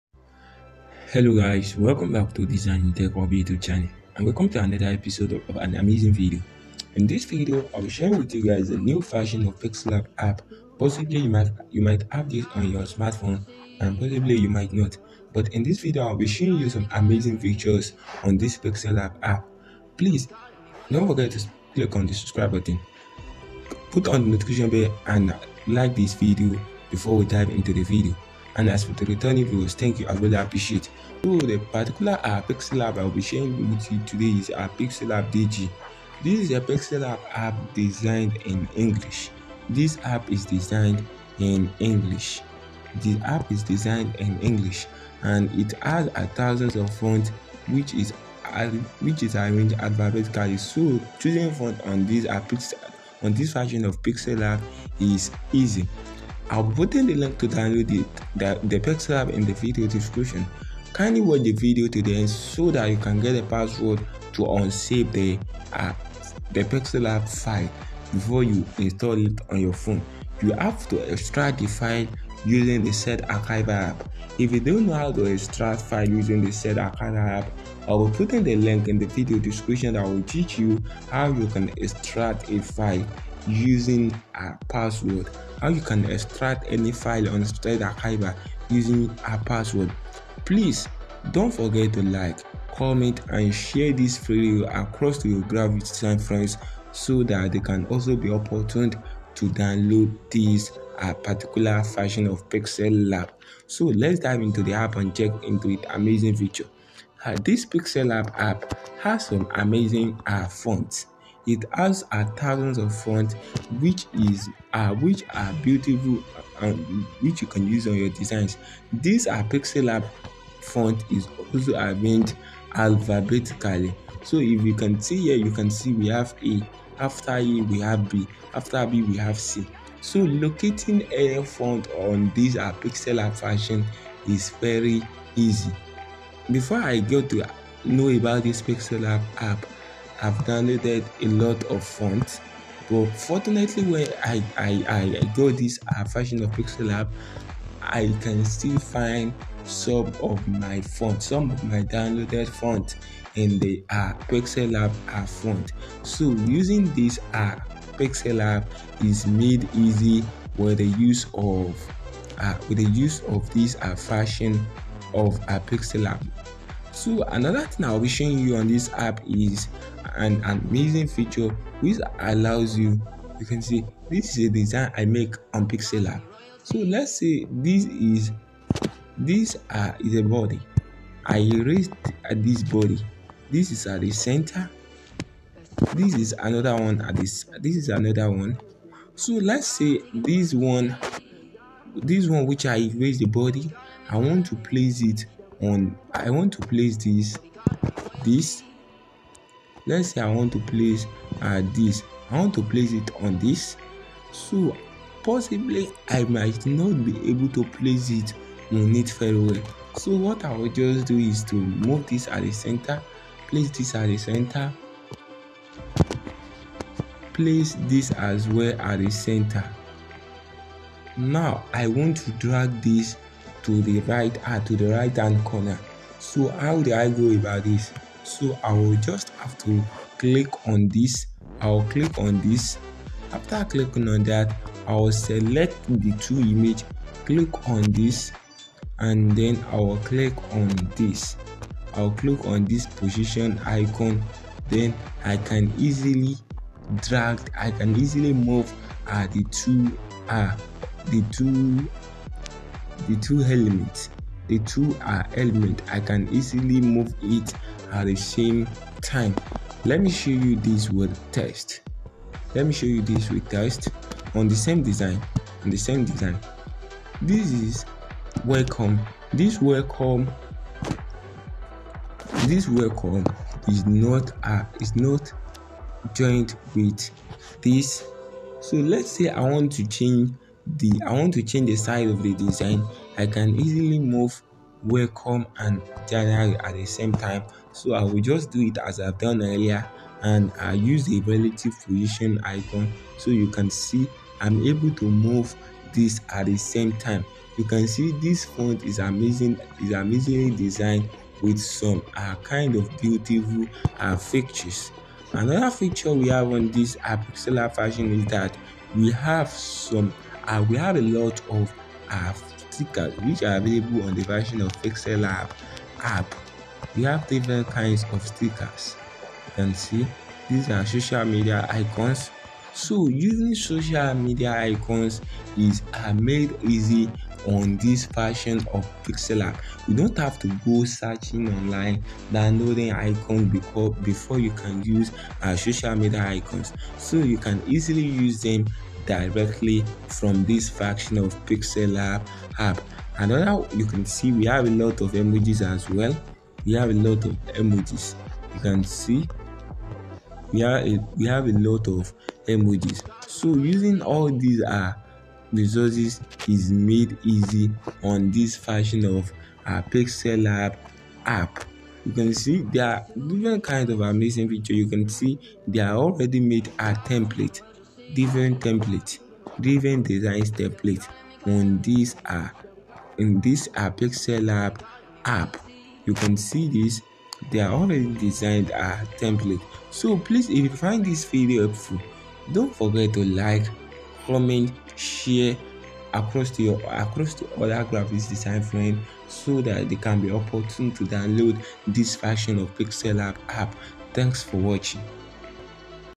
Download Pixellab DG🥇 || English Version | full Font || Free Mod Apk
In this video, I will be sharing Pixellab DG🥇|| latest version || full font || full template logo || amazing stickers || free size. Pixellab application (APK) are a great tool to work as a graphic designer and, more importantly, for you Graphics Designers to work without laptop (PC) or even Photoshop. Hope you enjoy the video, if you have any questions, please don't hesitate to leave them in the comments.

📌NOTE:- The App file is Password Protected🔑🔑, Watch the video carefully to get the password

📌FEATURE OF THE NEW APP
✨Premium Apk
✨+3k Fonts
✨Font arranged alphabetically
✨Logo Fonts
✨Decorative Fonts
✨Many stickers
✨Ultra save HD
✨ Attractive Look
✨No bug eraser
✨No bug stroke
✨No bug stroke emboss

.LET CONNECT.

📌GET DESIGN RESOURCES

ENJOY DESIGNING WITH  PIXELLAB PREMIUM 🤗